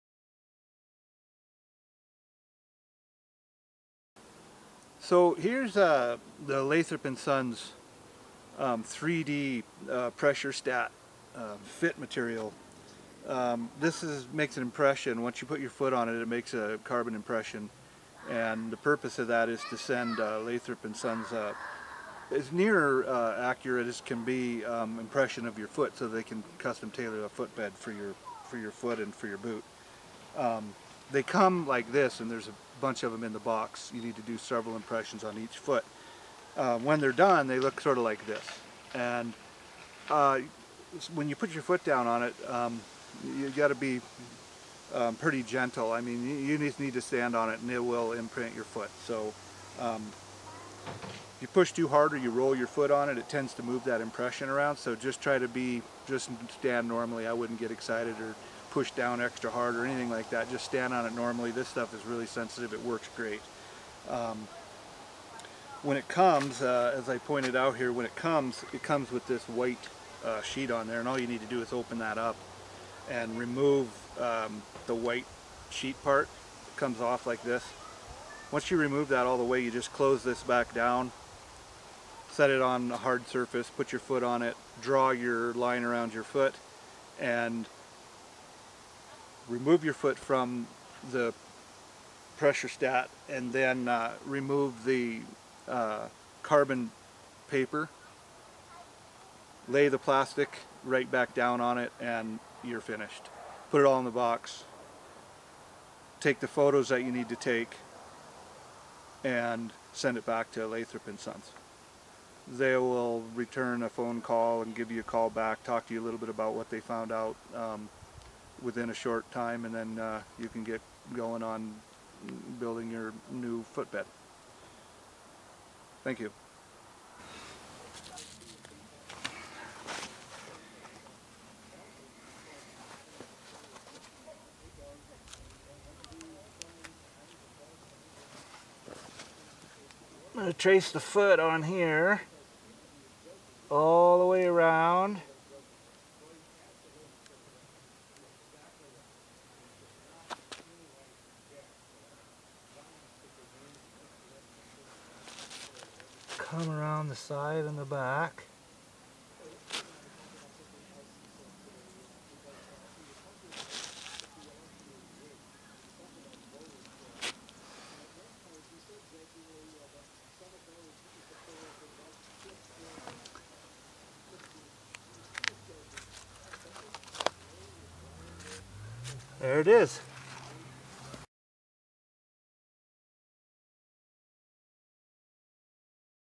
Tony Bynum Describes the Lathrop and Sons Custom Footbed 3D Mapping
Lathrop and Sons Custom 3D Footbed Mapping